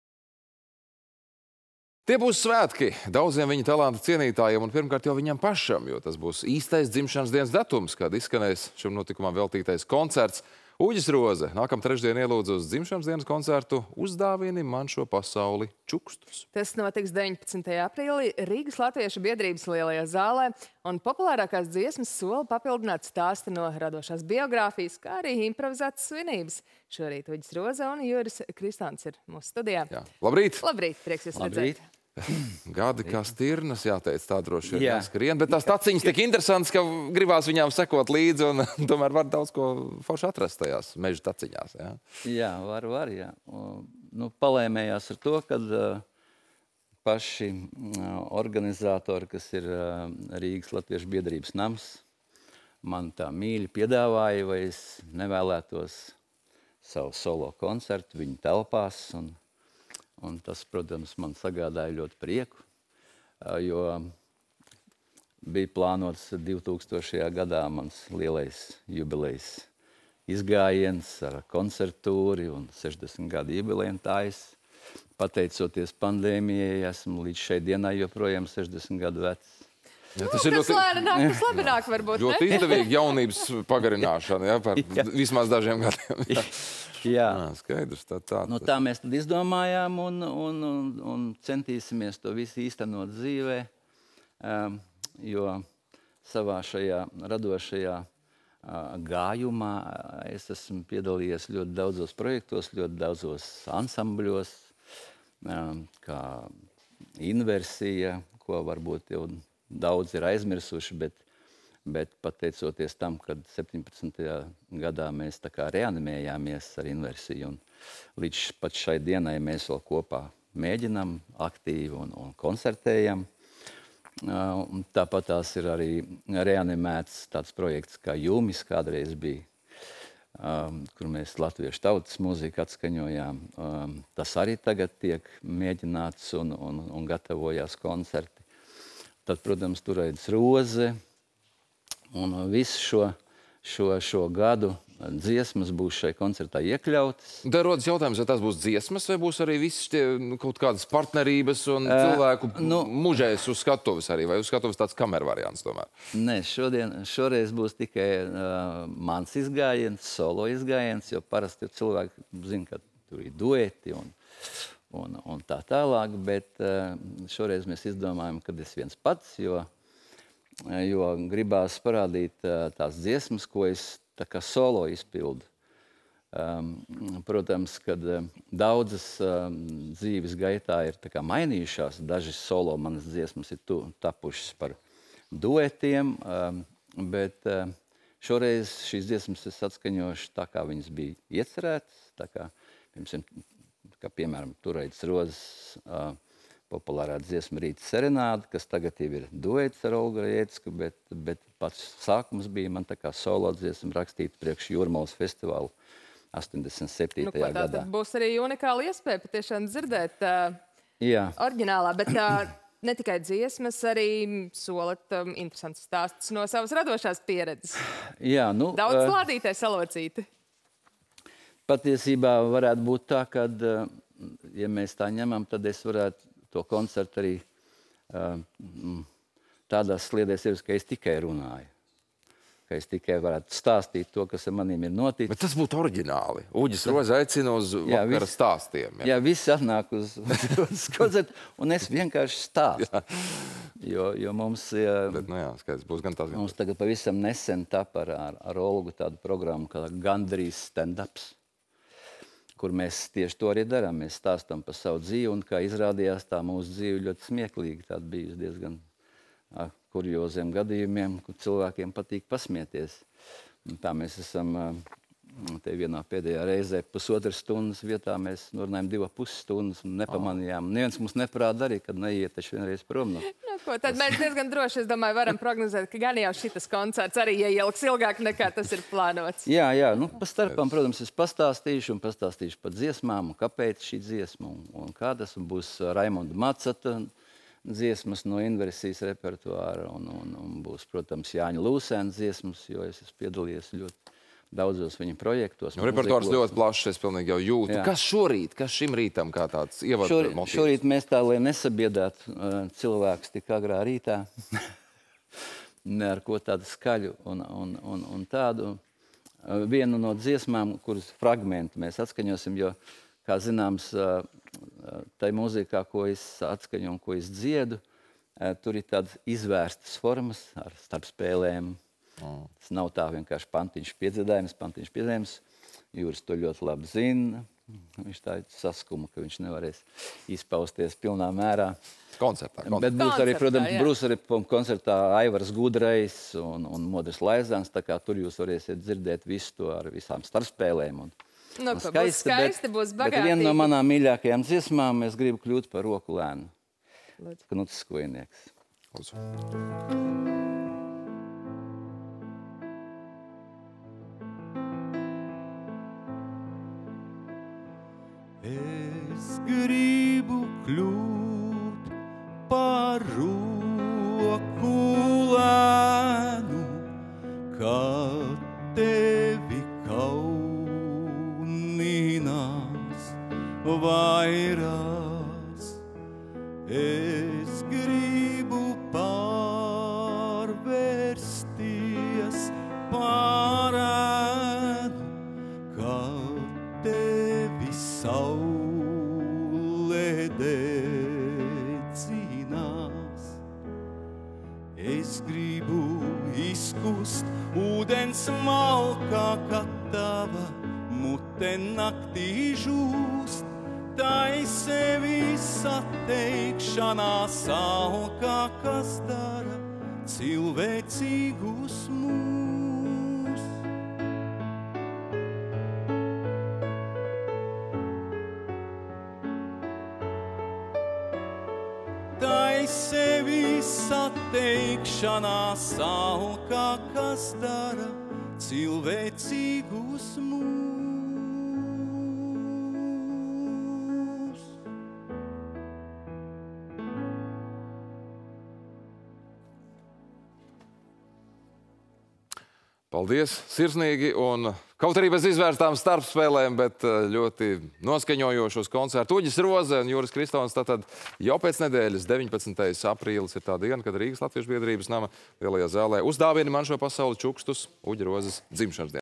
Dzimšanas dienas koncerts "Uzdāvini man šo pasauli čukstus"
Tie būs svētki daudziem viņa talanta cienītājiem. Un, pirmkārt, viņam pašam – jo tas būs īstais dzimšanas dienas datums, kad izskanēs šim notikumam veltītais koncerts. Uģis Roze nākamtrešdien ielūdz uz dzimšanas dienas koncertu "Uzdāvini man šo pasauli čukstus".
     Tas notiks 19. aprīlī Rīgas Latviešu biedrības Lielajā zālē, un populārākās dziesmas sola papildināt stāsti no radošās biogrāfijas, kā arī improvizētas svinības. Šorīt Uģis Roze un Juris Kristons mūsu studijā.